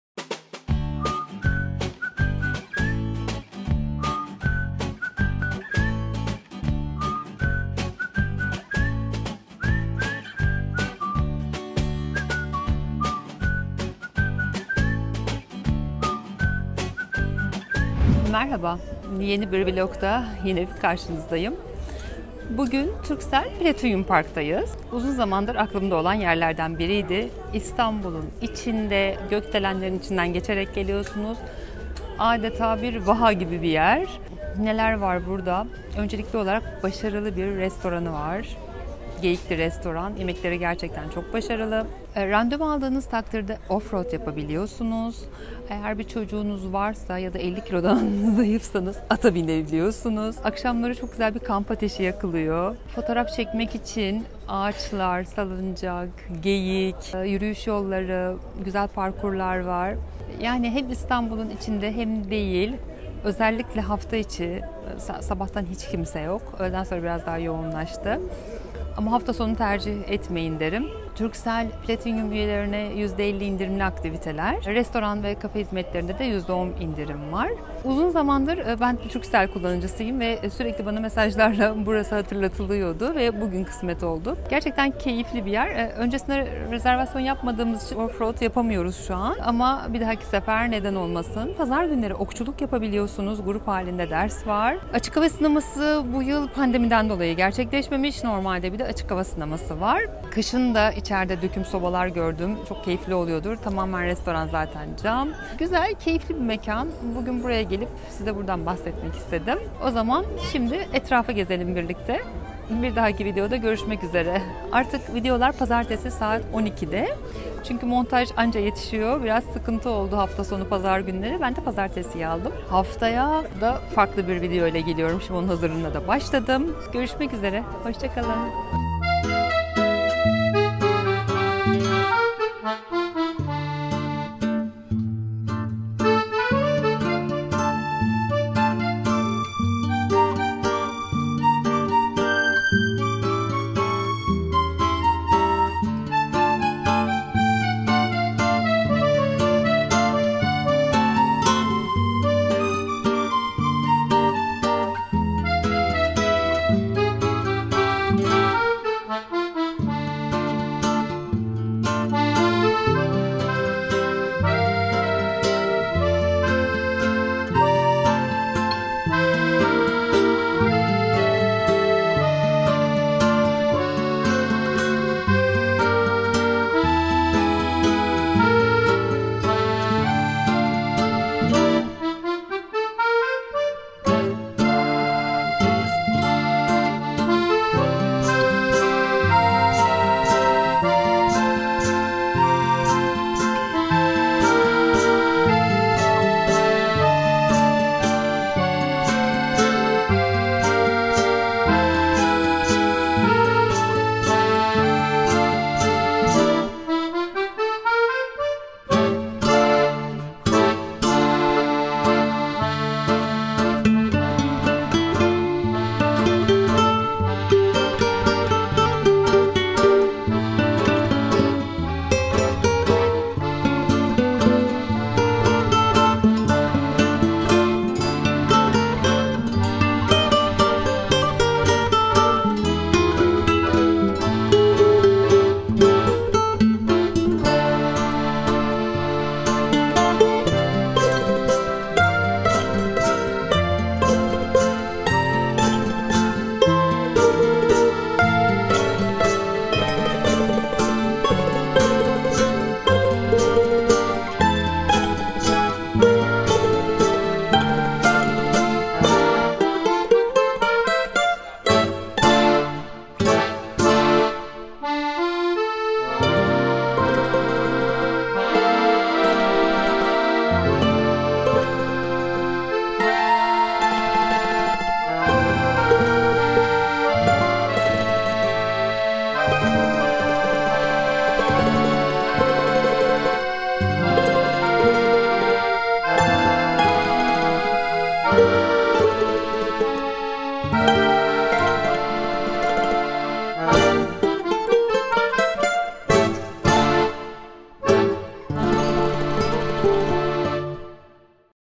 TURKCELL PLATINUM PARK
İstanbulluların doğayla buluşma noktası Türkcell Platinum Park'ta sakin, huzurlu bir gün geçirip, sizi de buradan haberdar etmek istedim.

Restoranı, geniş çocuk oyun alanları, yürüyüş parkurları, offroad, okçuluk ve atçılık aktiviteleri ile tercih edip memnun kalabileceğiniz bir yer.